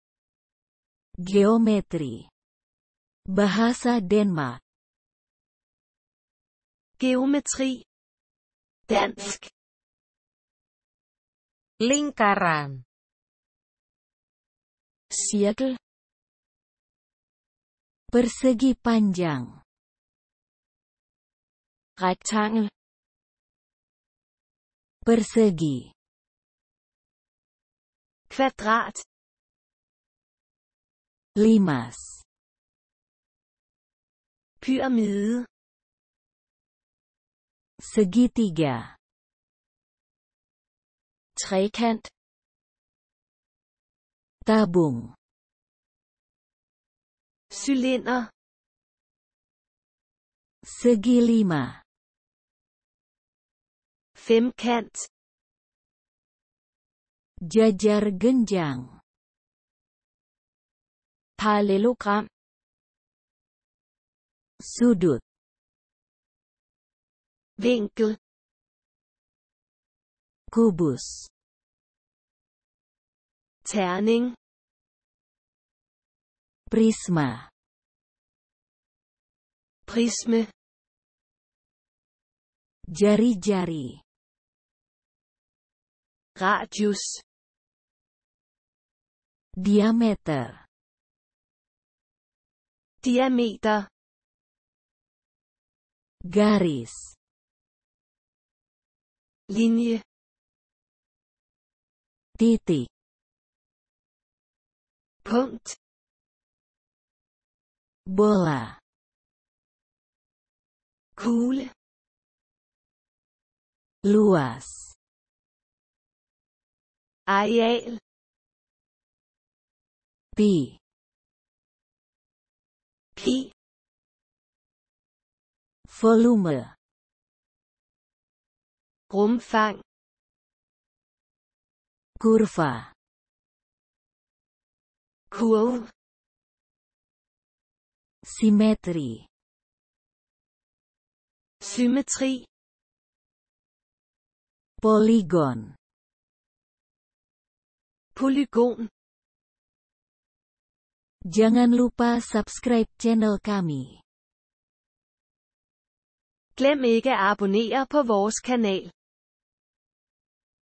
Geometri (Bahasa Denmark) (id-da)
Fone TV
id-da-10
Geometri (Dansk)
Lingkaran - Cirkel
Persegi panjang - Rektangel
Persegi - Kvadrat
Limas - Pyramide
Segitiga - Trekant
Tabung - Cylinder
Segi lima - Femkant
Jajar genjang - Parallelogram
Sudut - Vinkel
Kubus - Terning
Prisma - Prisme
Jari-jari - Radius
Diameter - Diameter
Garis - Linje
Titik - Punkt
Bola - Kugle
Luas - Areal
Pi - Pi
Volume - Rumfang
Kurva - Kurve
Simetri - Symmetri
Poligon - Polygon